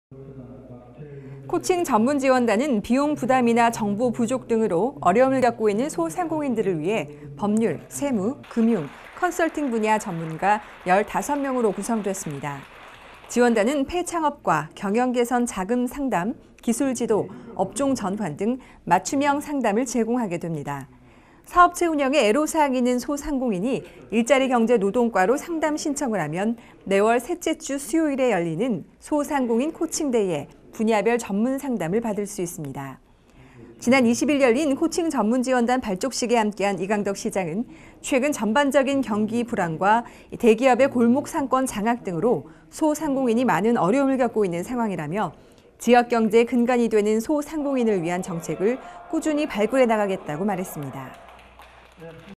소상공인 코칭전문지원단 위촉식
‘코칭전문지원단’은 비용부담이나 정보부족 등으로 어려움을 겪고 있는 소상공인들을 위해 법률, 세무, 금융, 컨설팅분야 전문가 15명으로 구성됐습니다.

지원단은 폐·창업과 경영개선 자금상담, 기술지도, 업종전환 등 맞춤형 상담을 제공하게 됩니다.

사업체 운영에 애로사항이 있는 소상공인이 일자리경제노동과로 상담신청을 하면 매월 셋째 주 수요일에 열리는 ‘소상공인 코칭-Day'에 분야별 전문 상담을 받을 수 있습니다.

지난 20일 열린 ‘코칭전문지원단 발족식’에 함께 한 이강덕 시장은 “최근 전반적인 경기불황과 대기업의 골목상권 장악 등으로 소상공인들이 많은 어려움을 겪고 있는 상황”이라며,
“지역 경제의 근간이 되는 소상공인을 위한 정책을 꾸준히 발굴해 추진해 나가겠다”고 말했습니다.